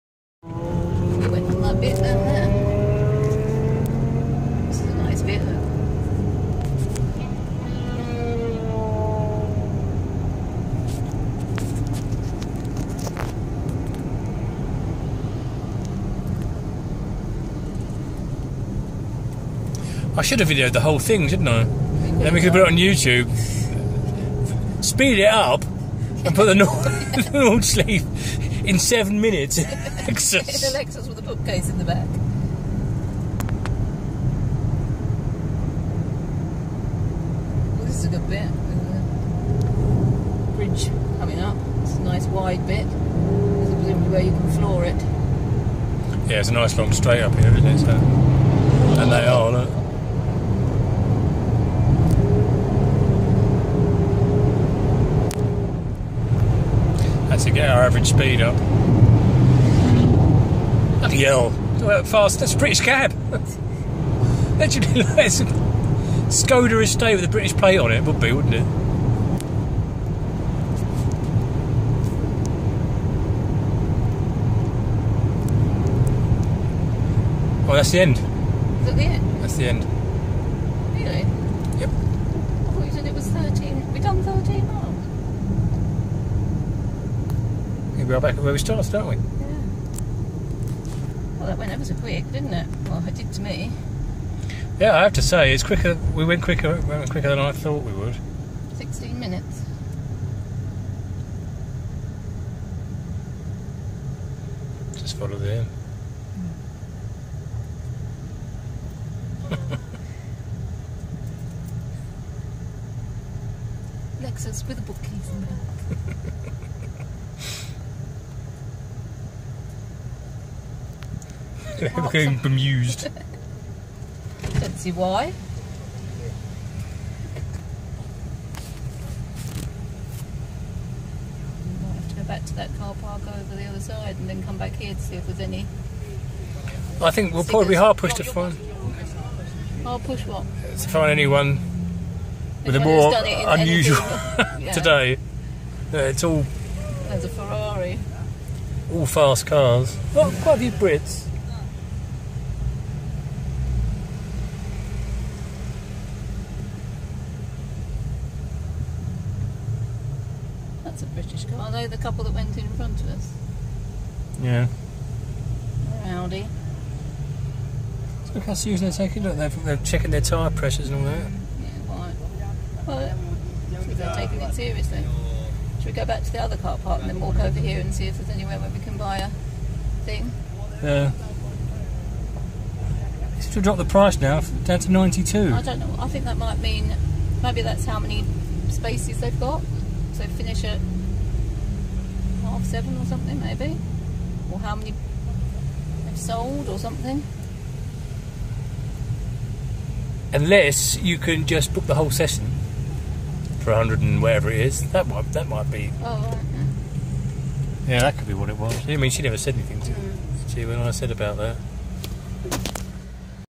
Lap of the Nordschleife in a Lexus RX, with dodgy brakes, and a Bookcase in the back!
We visited the Nurburgring on a roadtrip down through Germany, on our way to our holiday home (hence the bookcase), and my partner just had to have a go...